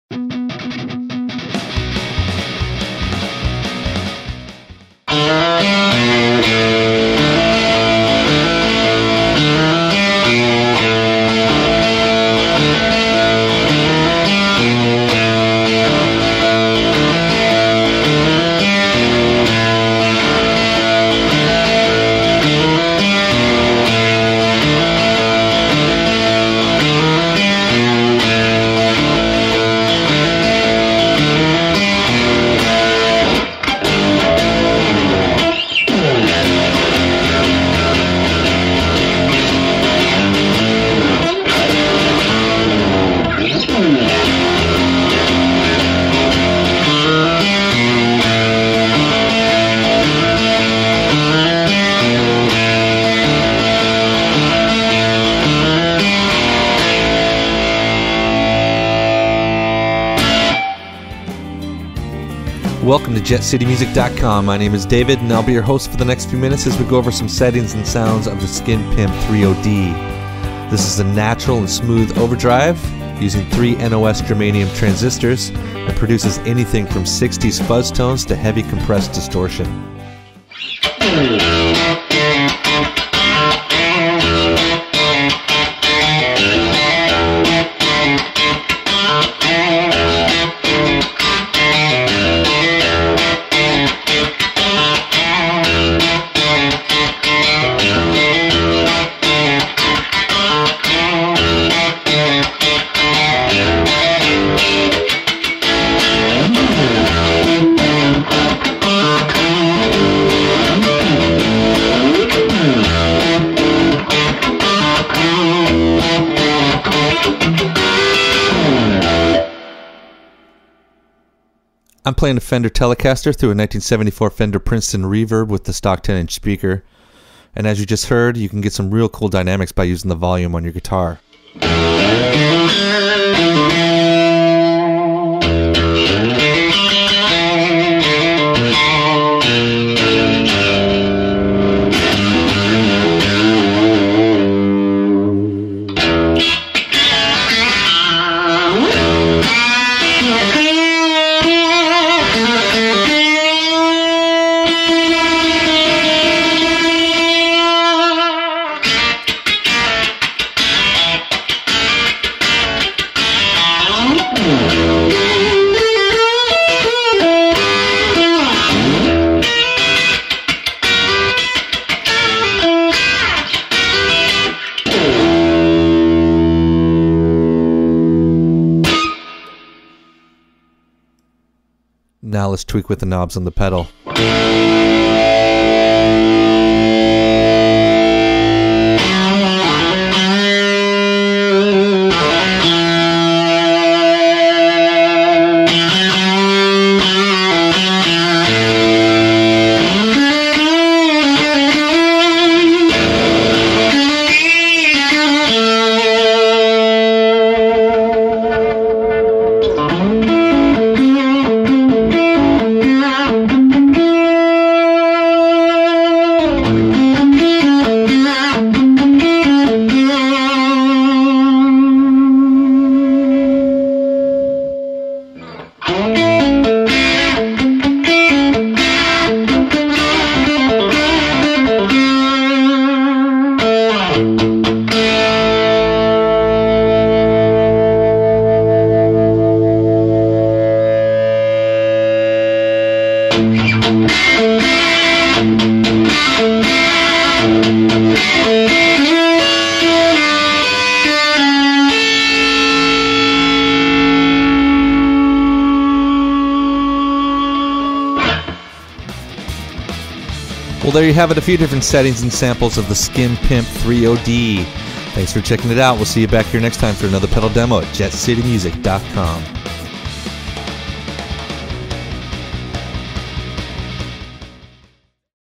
Skinpimp 3OD
The 3OD is a low to mid gain overdrive that feels like a natural extension of your guitar to your amp. It has plenty of level on tap to push a slightly broken up amp into tube saturated bliss. This is truly a versatile overdrive pedal with almost endless tonal possibilities. Available at JetCityMusic.com.